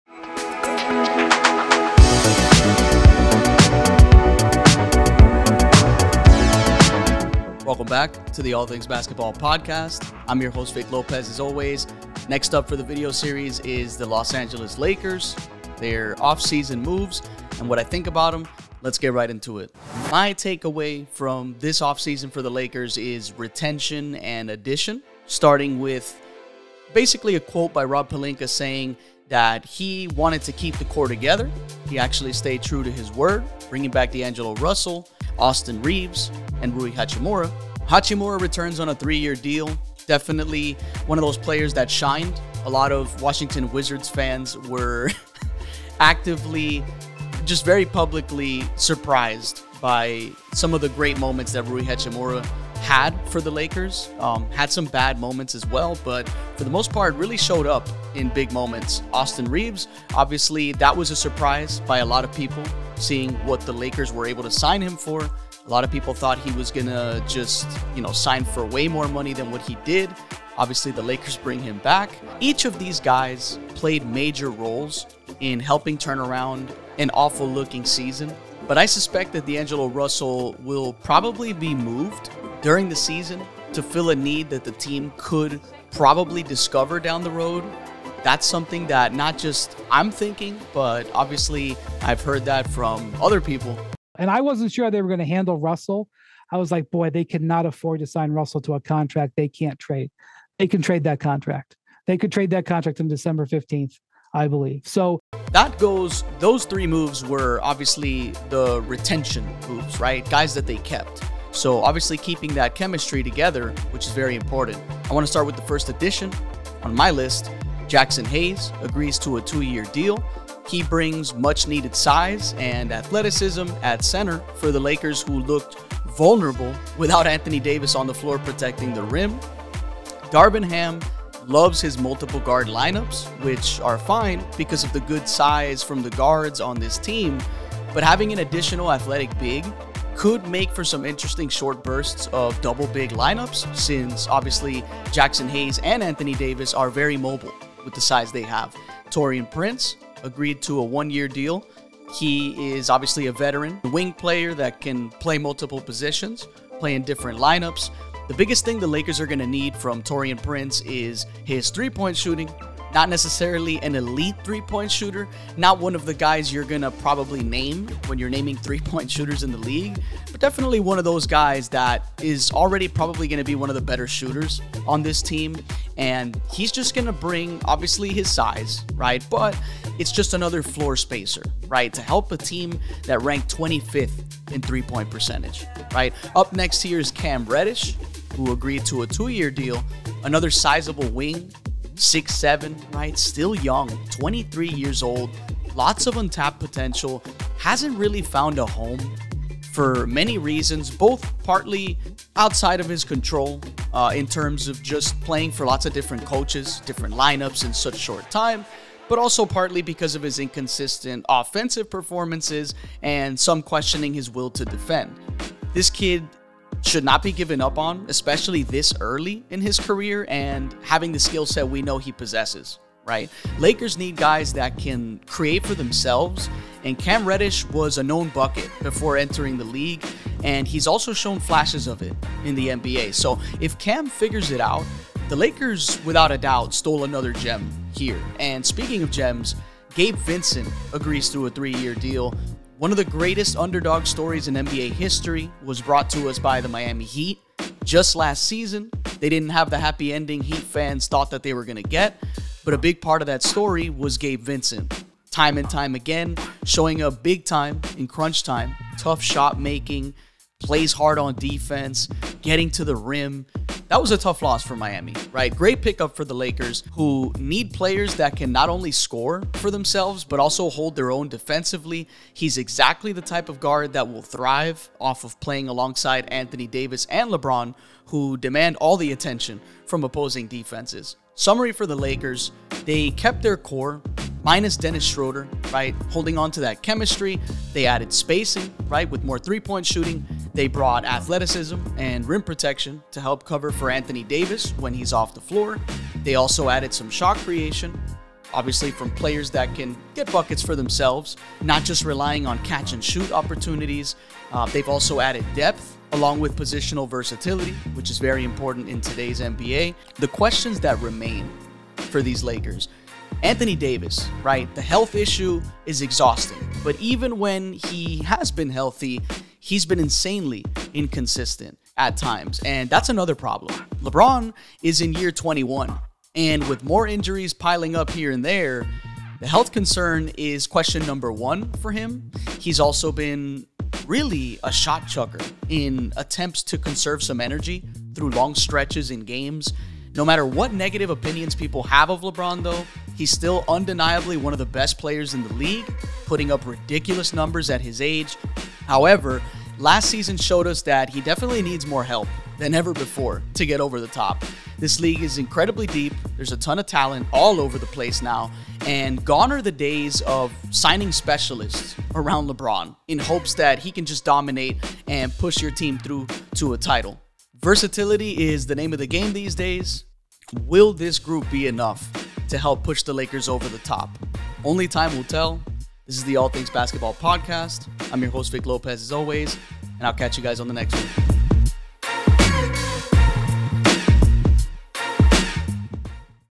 LOS ANGELES LAKERS OFF-SEASON THOUGHTS
Up next on my off-season thoughts video series are the Los Angeles Lakers! A team that was able to keep some continuity along with adding more to their squad in hopes for another deep run next season!